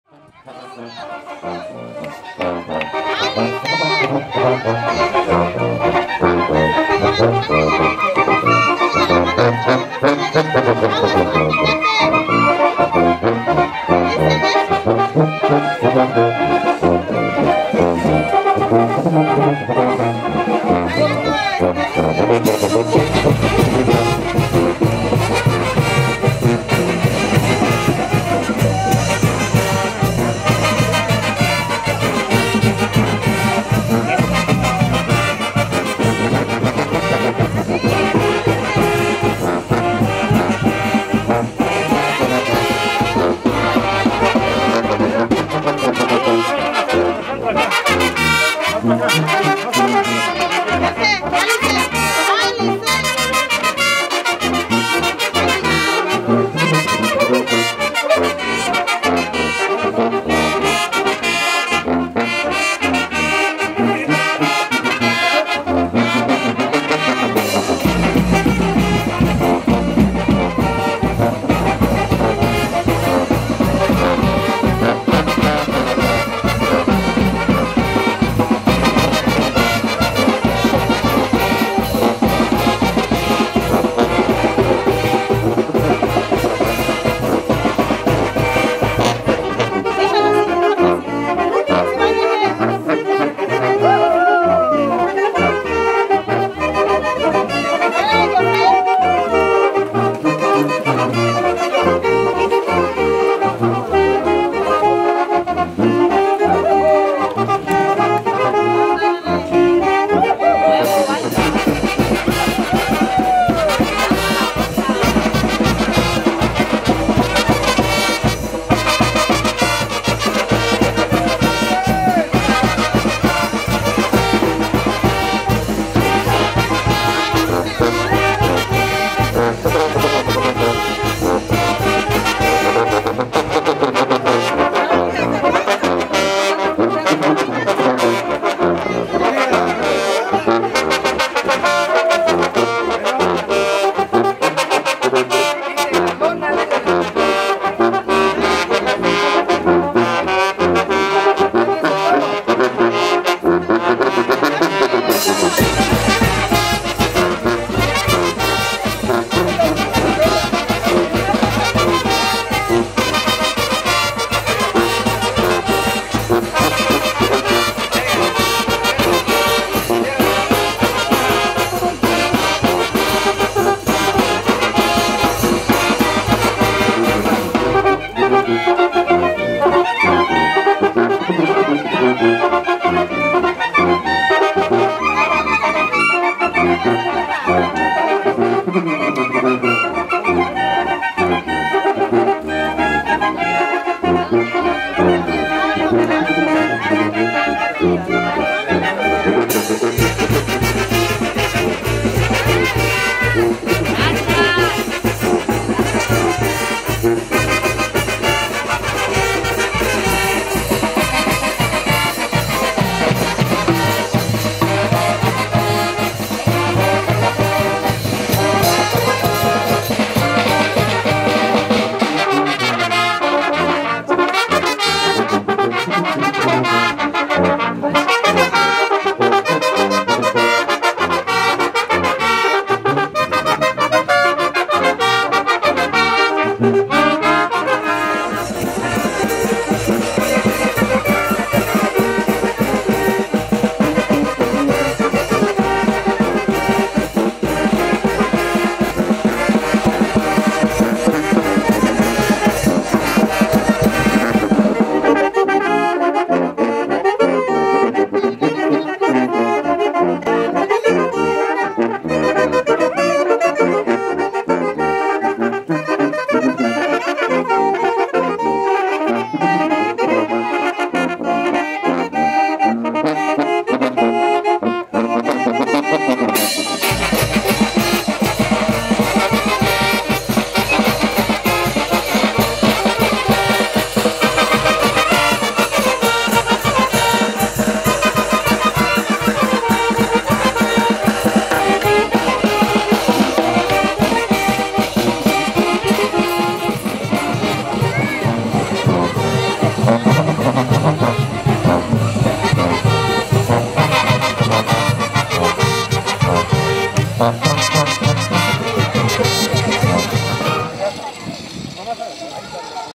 Carnaval de guerrero sta. Cruz Itundujia Con la Participación Del KINDER CENTRO 2017
Carnaval de guerrero Santa Cruz Itundujia 2017